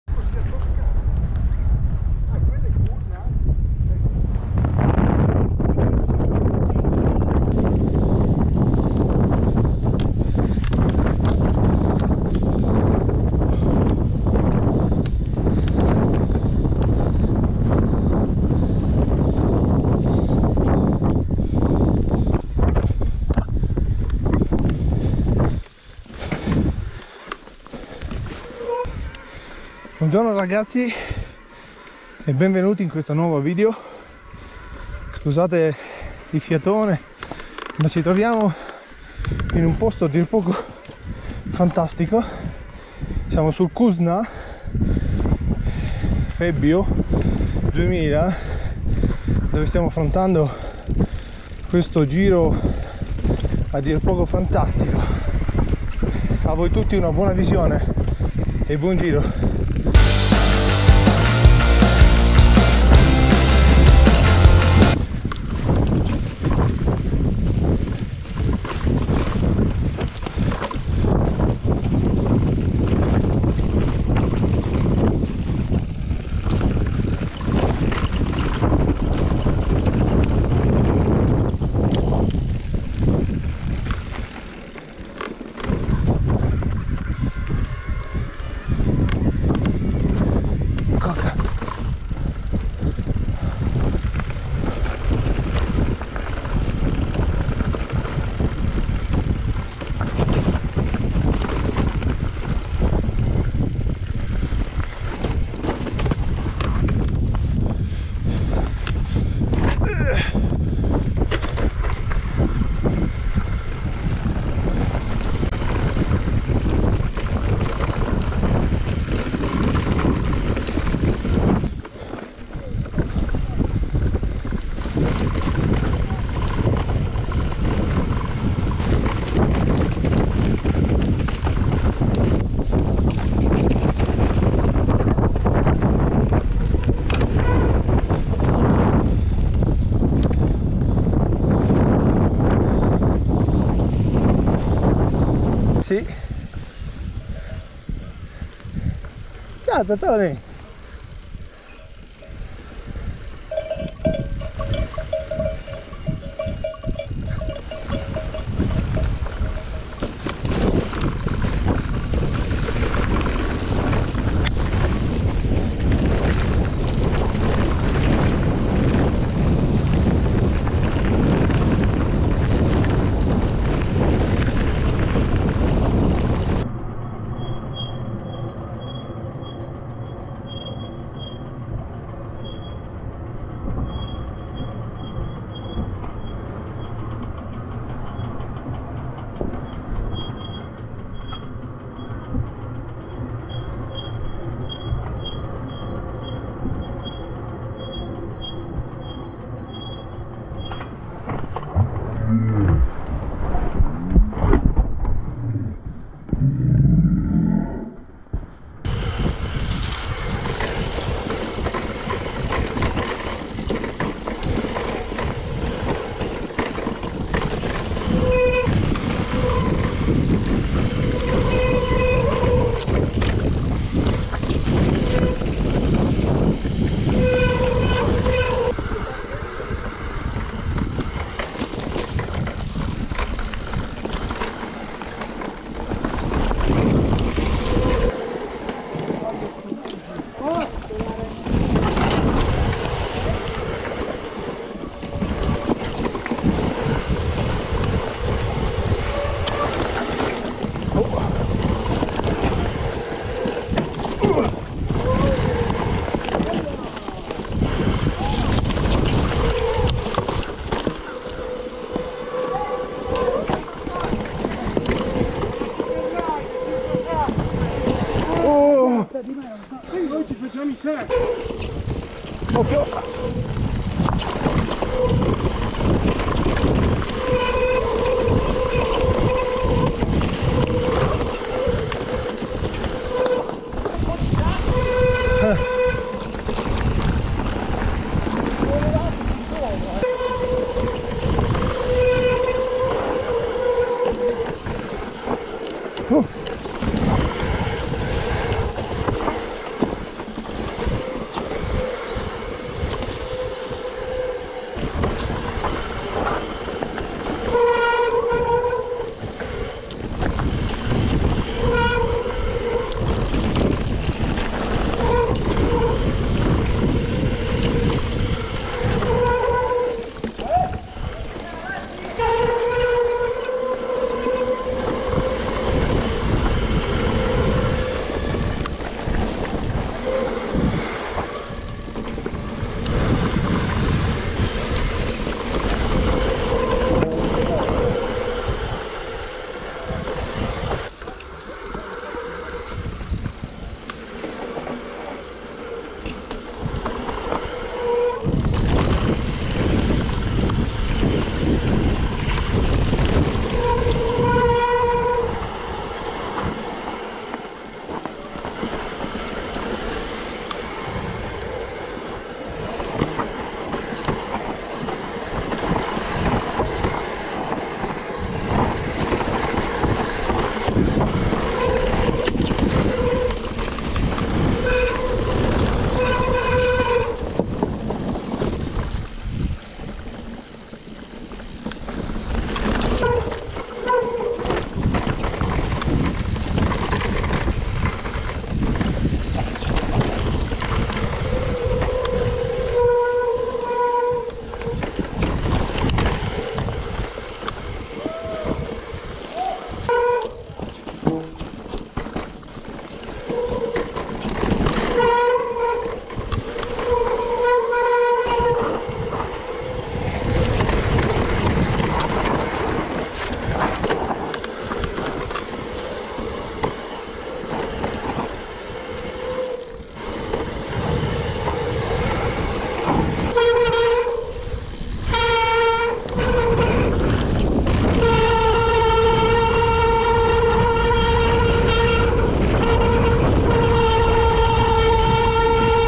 Monte Cusna febbio mtb
Giro all'interno del comprensorio del cusna  febbio Reggio Emilia